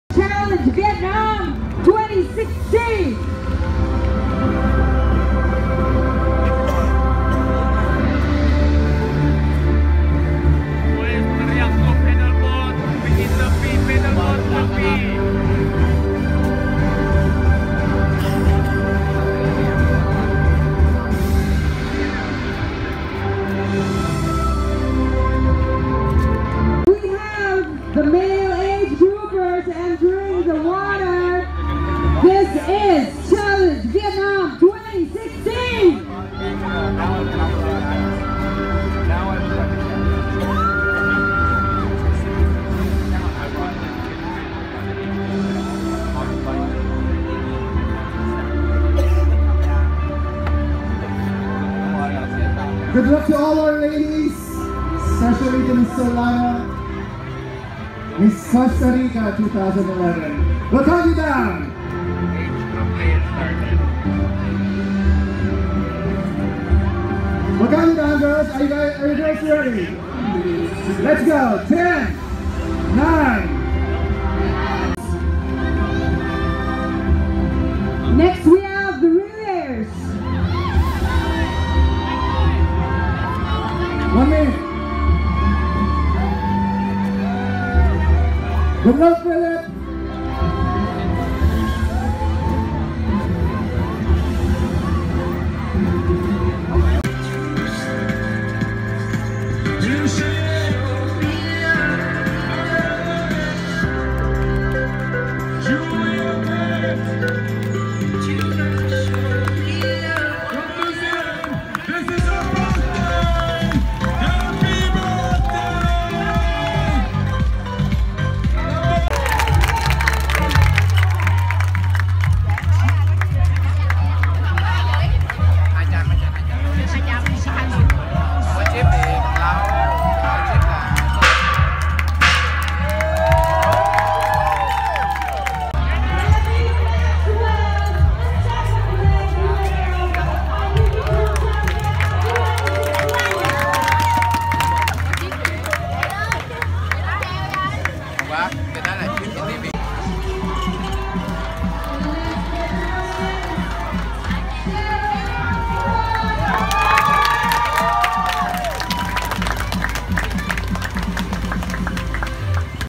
Challenge Triathlon Viet Nam 2016 in Nha Trang!
Challenge Family, with more than 40 races all over the world, had its first race in Vietnam on September 11, 2016. Athletes from over 30 countries, converged in Nha Trang to face one of the biggest challenges of the year, 1.9km Swim, 90km Bike and 21km Run. This year, Challenge Vietnam after a great success and being nominated as the best inaugural triathlon event in Asia, it will be back on August 27, 2017.

Situated in the South Coast of Vietnam, Nha Trang is known as the capital beach of the country. By coming to Nha Trang, both athletes and families will be able to enjoy the numerous tourist attractions, to taste delicious fresh seafood and to get to know a new and completely different culture. Moreover, its geography and weather conditions offer the possibility of having a remarkable course and enjoying a great race vacation.